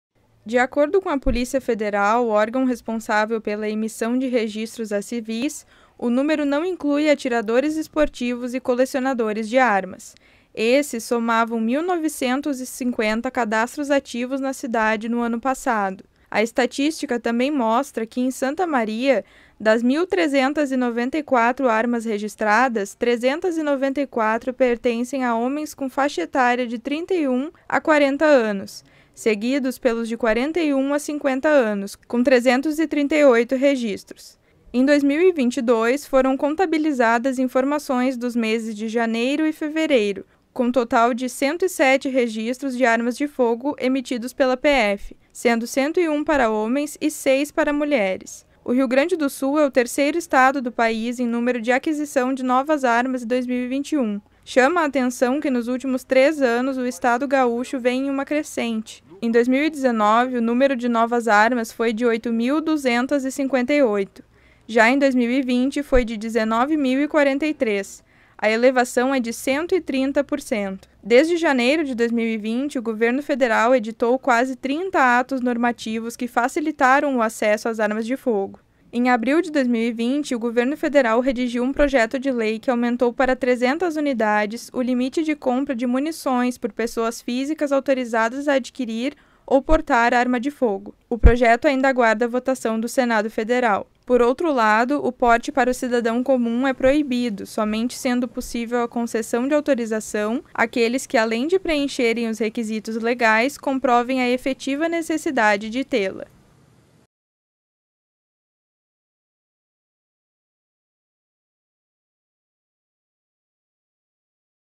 Mais de 1,3 mil armas de fogo foram registradas em 2021
Os homens entre 31 e 50 anos são os mais interessados em ter posse de revólveres e pistolas no município